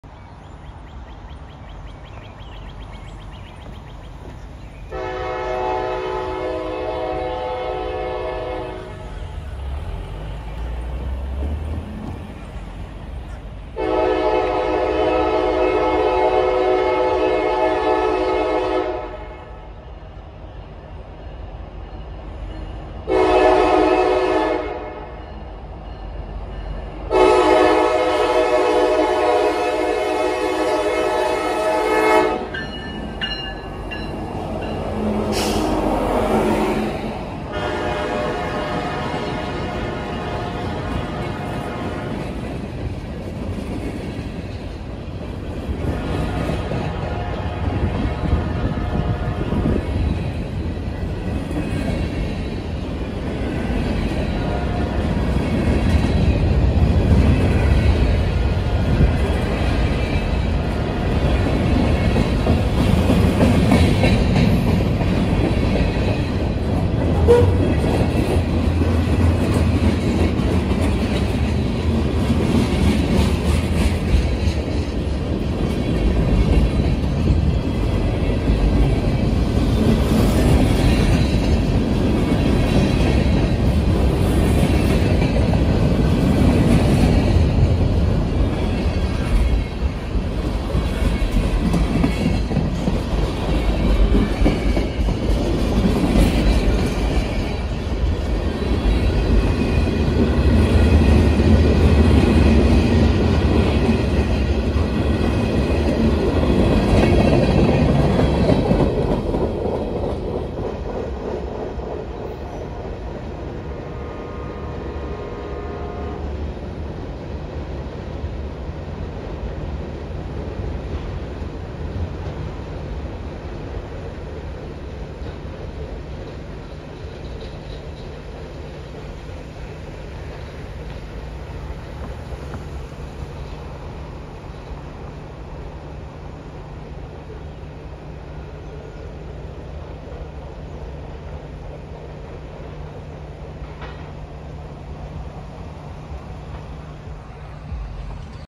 Norfolk Southern H95 - 4/18/24
Norfolk Southern local freight train H95 is seen crossing Walnut Street on the York Secondary with a Nathan K5LA horn in Mount Wolf, Pennsylvania at 6:50 AM on Thursday, April 18, 2024. The engines in this video are: NS 6977 (SD60E) and NS 6933 (SD60E). Enjoy!